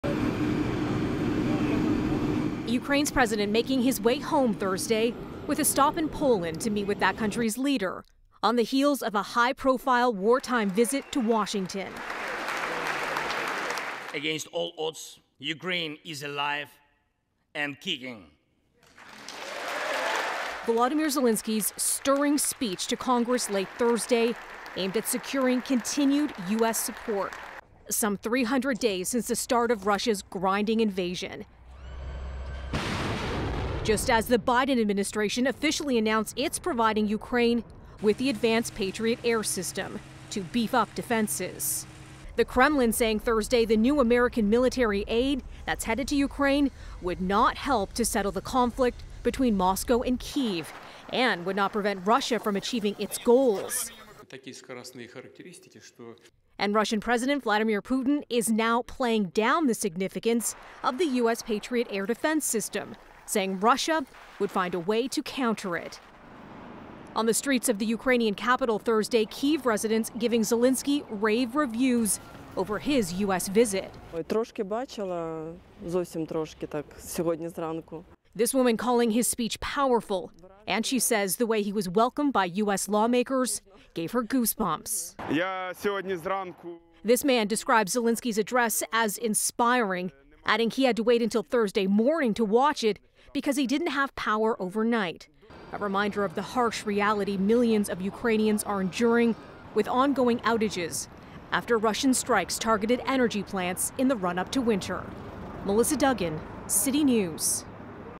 Zelenskyy touts ‘good results’ from historic U.S. trip
How is Moscow reacting to the Ukrainian president’s high-profile trip to Washington? Melissa Duggan on the Kremlin’s dismissal of new U.S. military support for Kyiv.